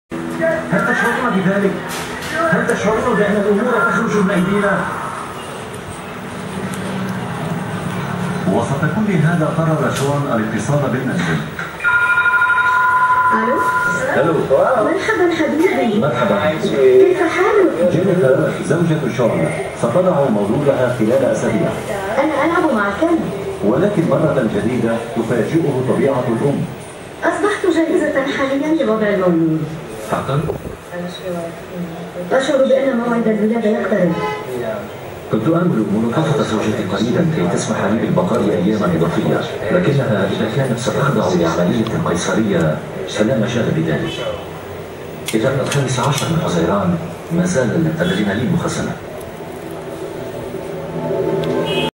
(835) Foods and Beverage in Hurghada ,Egypt 2 (HD)
Here is a local good restaurant where my friend tell me in Hurghada  :-D
The taste　of  the foods are really  egyptian flavor   :-)
They are famous "Maḥshi ḥamām", It's a   roasted Pigeon that stuffed with rice or wheat and herbs and the very healthy  "Molokheyyah soup" and so on =)
Thank you owner for a good egyptian dinner  ;-)